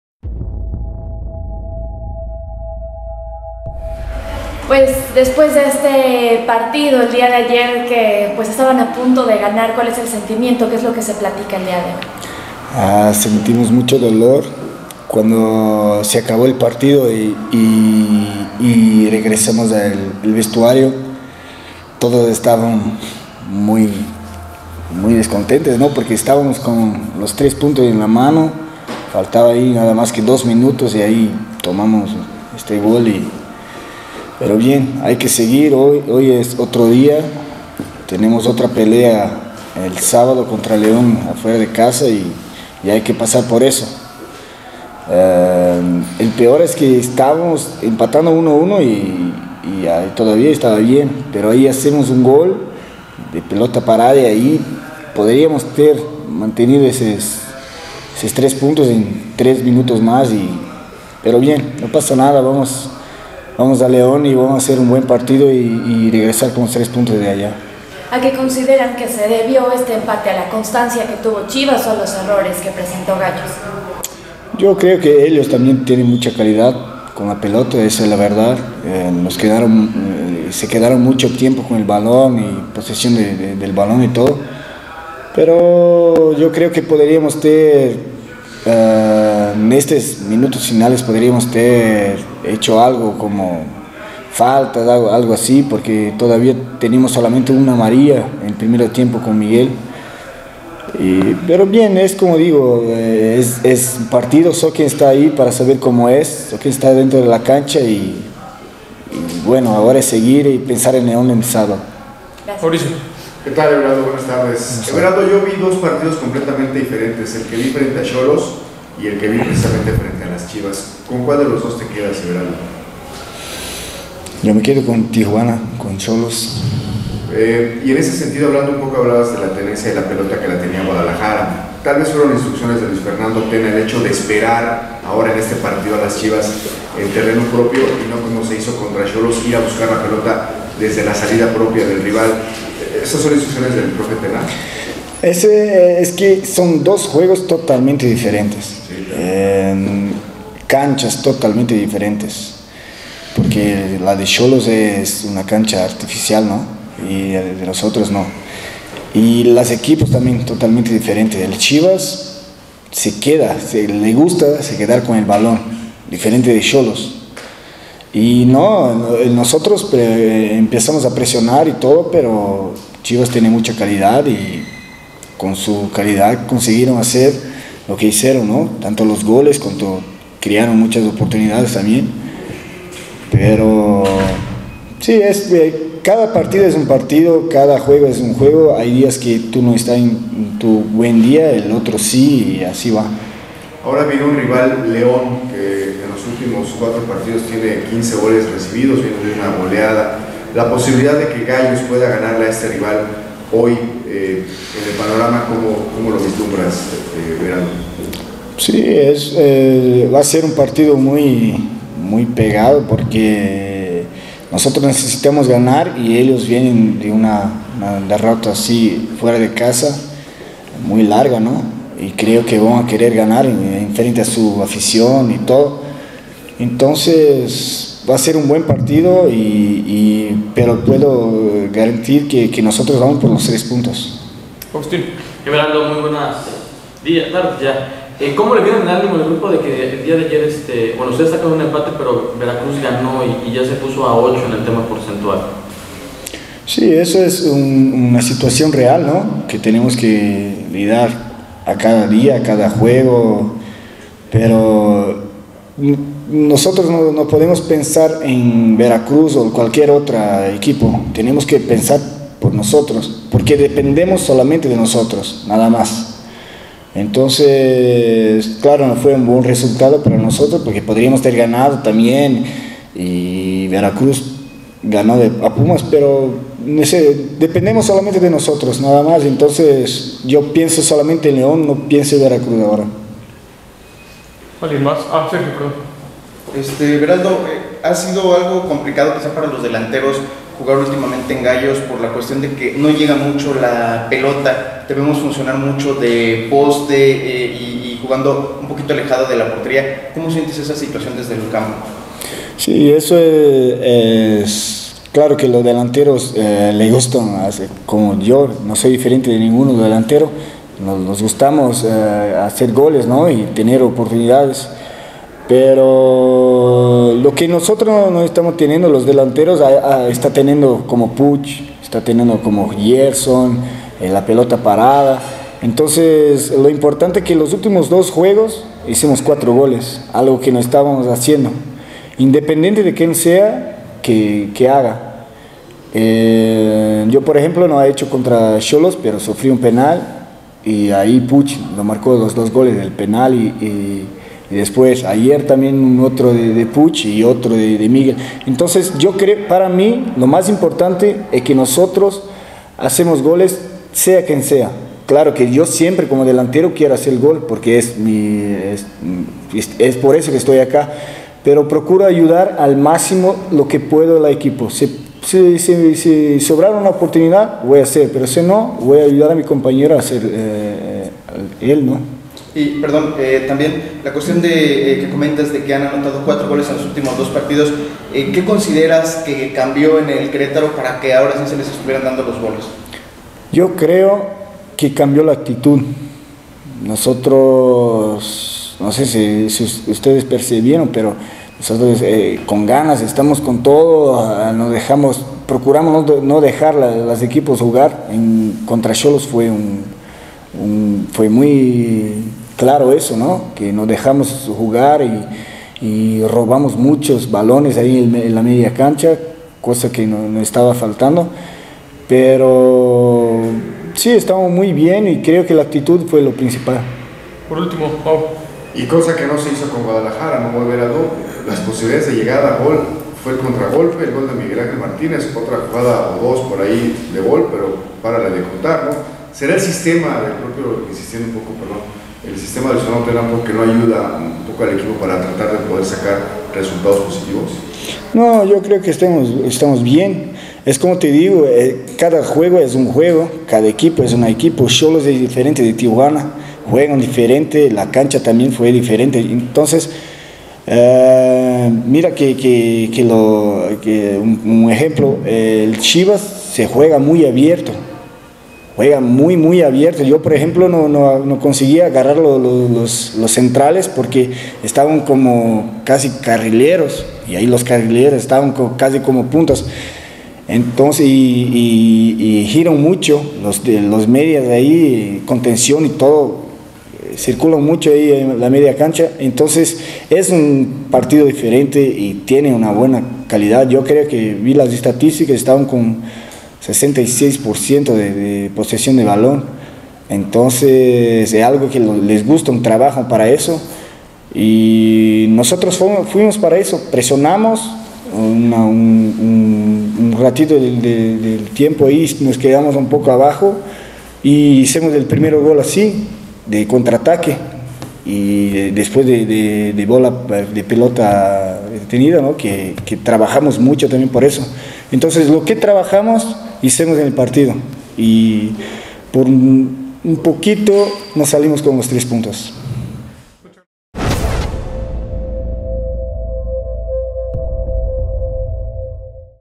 Everaldo Stum afirmó que el equipo ya está enfocado en León.
Stum destacó la mejoría de la ofensiva ante Chivas